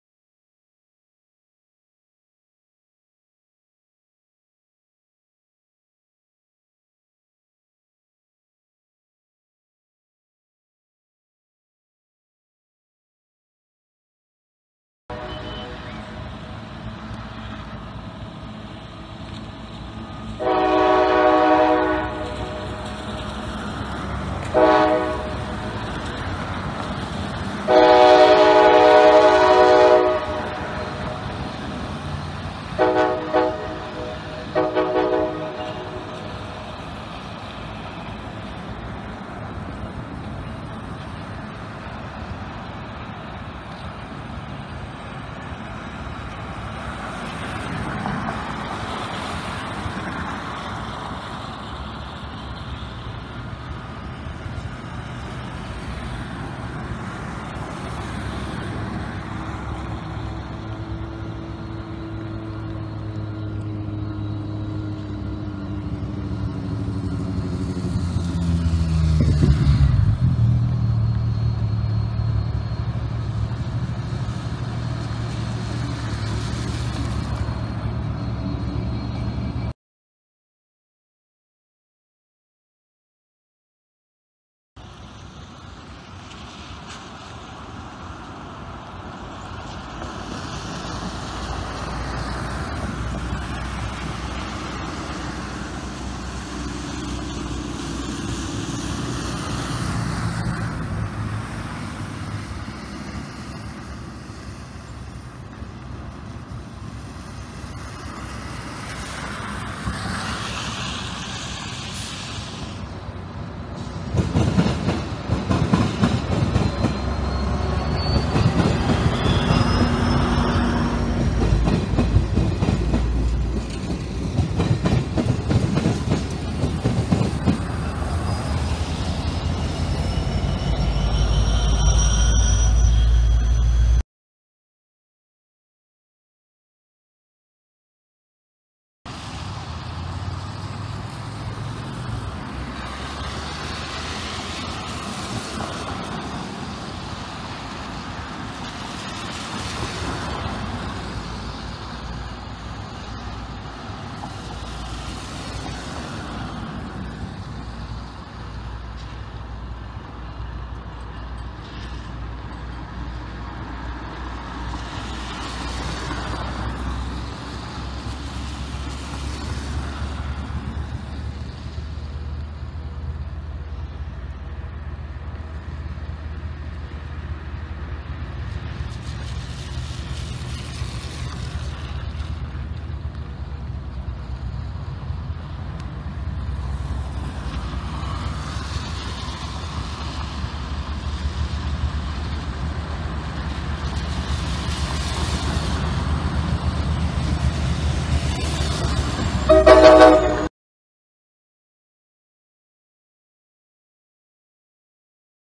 Raleigh NC Piedmont 75 Boylan Wye
Raleigh NC (RGH) Piedmont 75 arriving from the NCDOT Capital Yard, through Boylan Wye on CSX track, backs into Raleigh Cabarrus Street Station in 2017 before the new Union Station was opened. A late Silver Star 92 (looks like sleeper cars at the rear?) appears to be in the station in the first clip but has departed before 75 gets to the station to begin boarding for a 10 a.m. departure. Construction work on Union Station can be seen, as well as switches and areas dedicated to new station tracks.